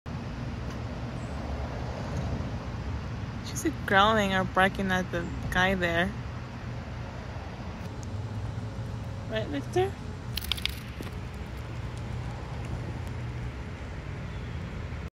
Minnie reminds me of Cosita 💕 🖤 Walking Minnie the Chihuahua and Victor the Yorkie
Minnie reminds me of Cosita 💕 🖤 Walking Minnie the Chihuahua and Victor the Yorkie at 12:30pm! HappyThursday!